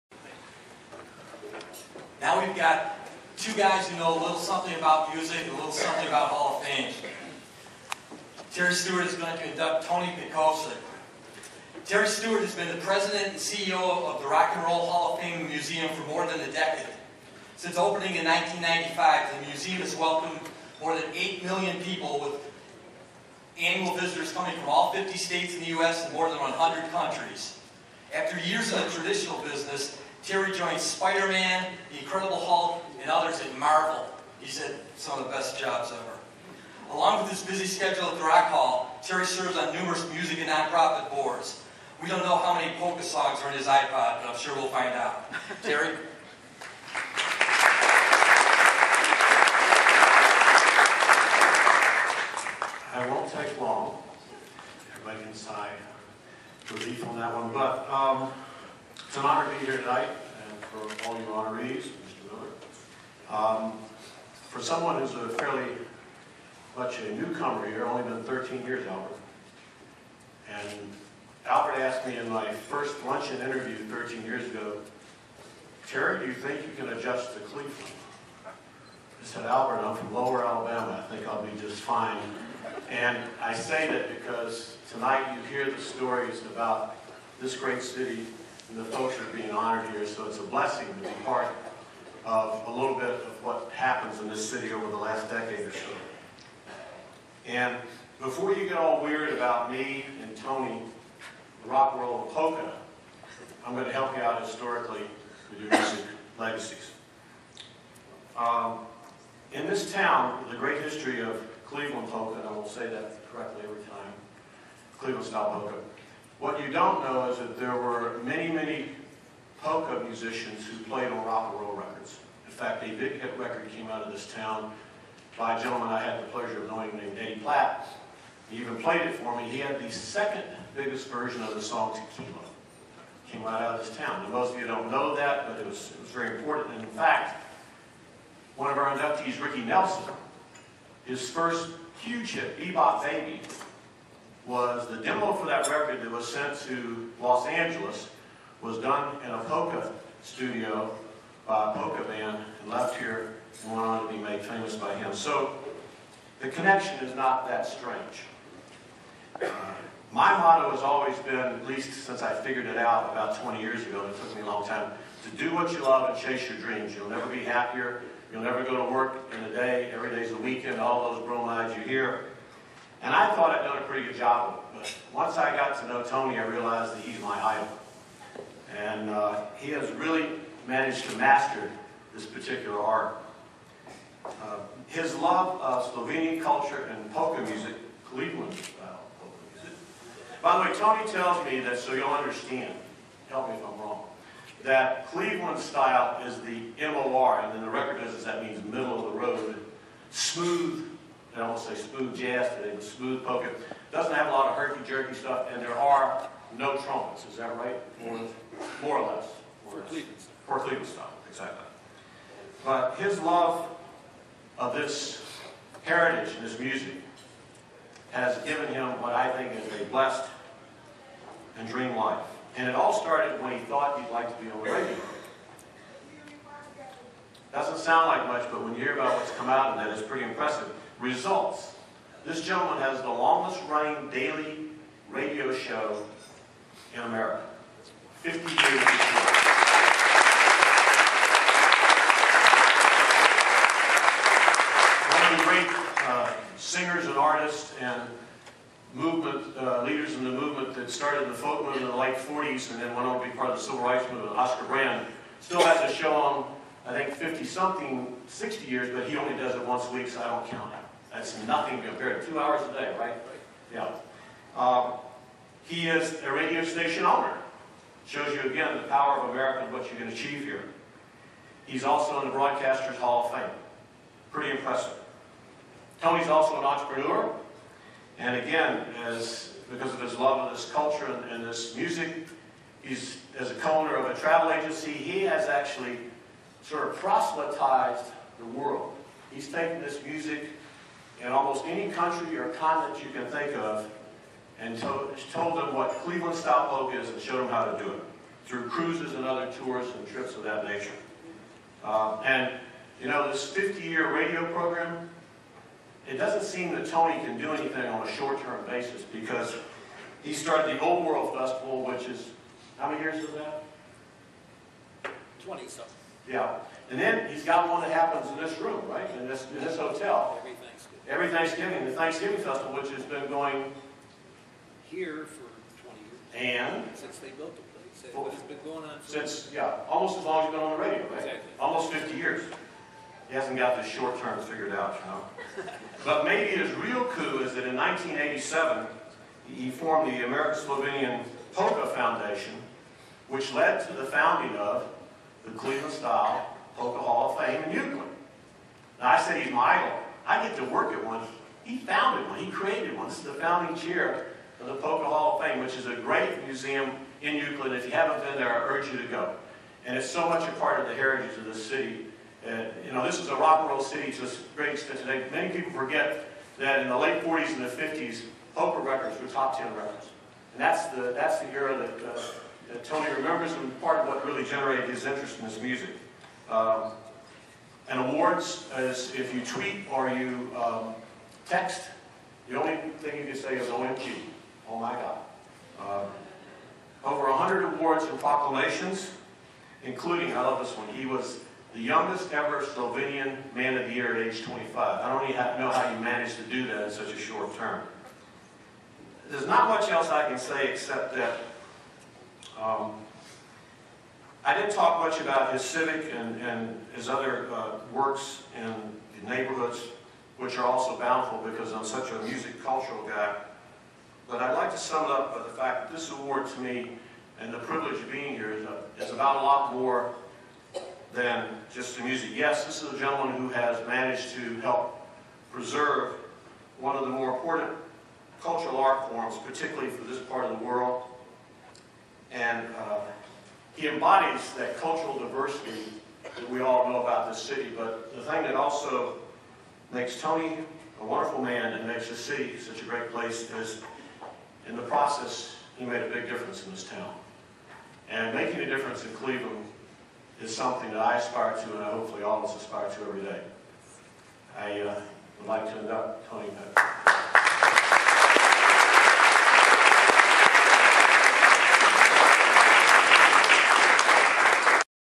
Rock Hall's Terry Stewart inducts Polka King
Terry Stewart, President and CEO of the Rock and Roll Hall of Fame and Museum, inducts Cleveland Style Polka King Tony Petkovsek induction into the Cleveland International Hall of Fame.